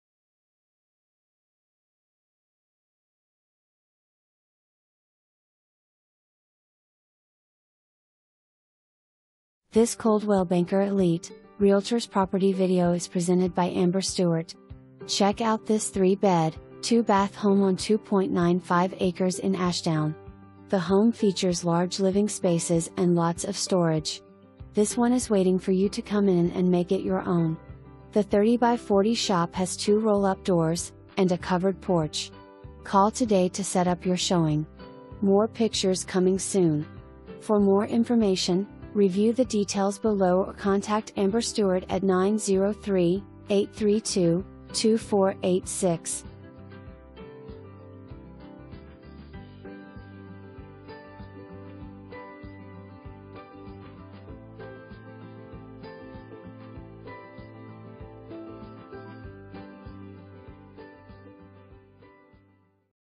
Coldwell Banker Elite, Realtors - 172 LR 27
Please enjoy this property video created by Coldwell Banker Elite, Realtors

172 LR 27
Ashdown, AR 71822

Check out this three bed, two bath home on 2.95 acres in Ashdown! The home features large living spaces and lots of storage. This one is waiting for you to come in and make it your own. The 30x40 shop has two roll up doors, and a covered porch. Call today to set up your showing! MORE PICTURES COMING SOON!

2 full bath

Amber Stewart
Coldwell Banker Elite